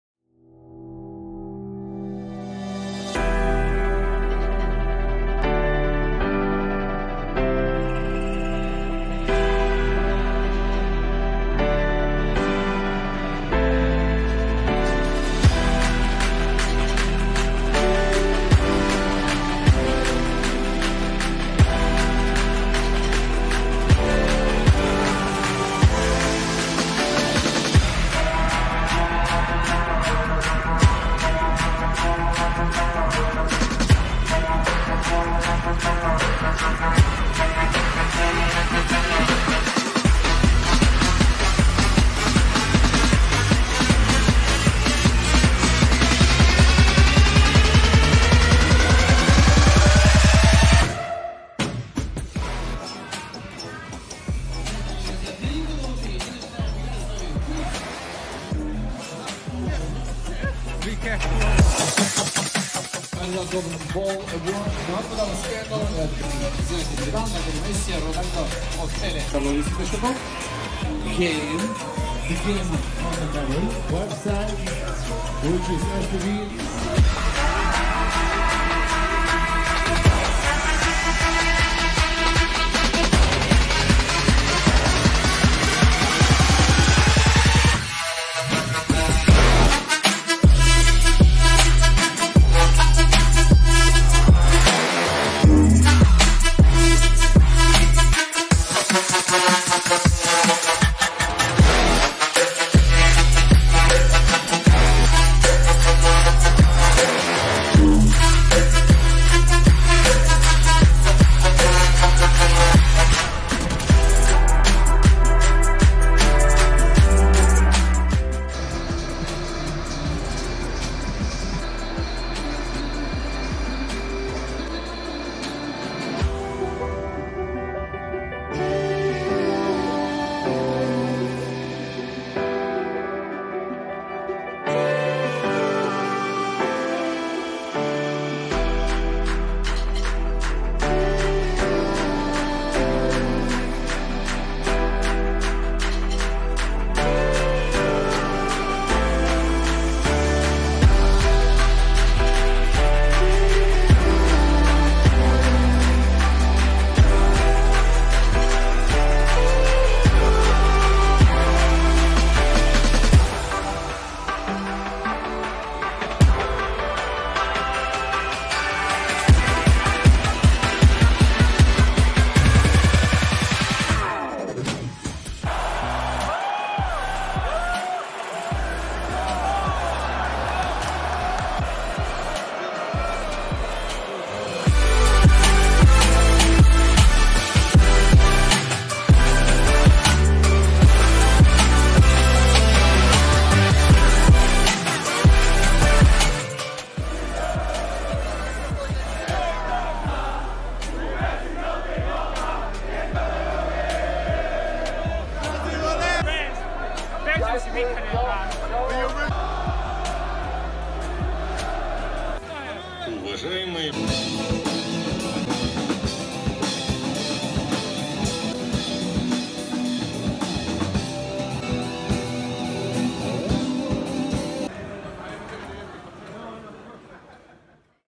PriveReview Semi Final France Belgium St Petersburg 10.7.2018
Kỷ niệm đáng nhớ khi lần đầu ghé sân vận động St. Peterburg xem trận bán kết Pháp - Bỉ tại FIFA World Cup Russia 2018